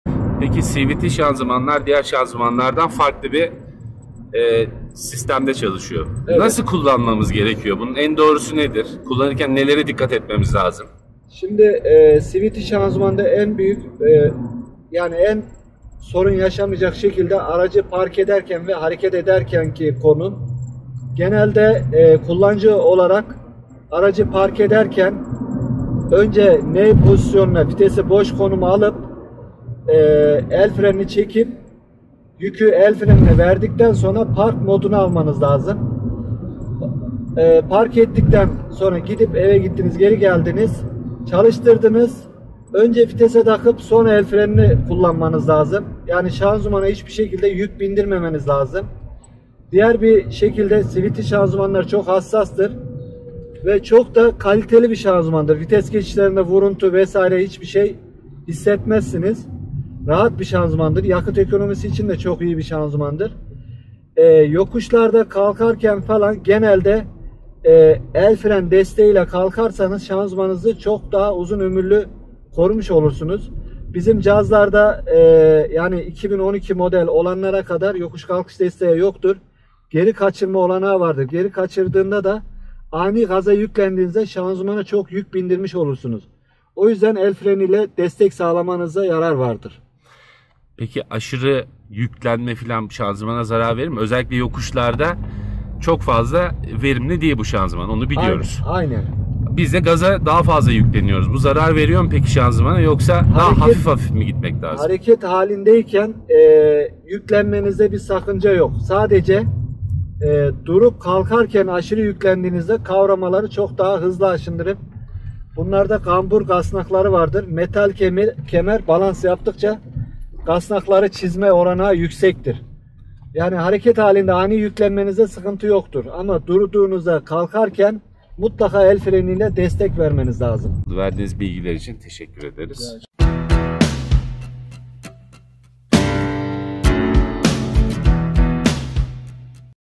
CVT ŞANZIMANLI ARAÇLAR NASIL KULLANILMALI? SAKIN BUNLARI YAPMAYIN! DOĞAN USTA CEVAPLADI..
CVT ŞANZIMANLI ARAÇLARI KULLANIRKEN NELERE DİKKAT ETMEMİZ GEREKİYOR DOĞAN USTA CEVAPLADI.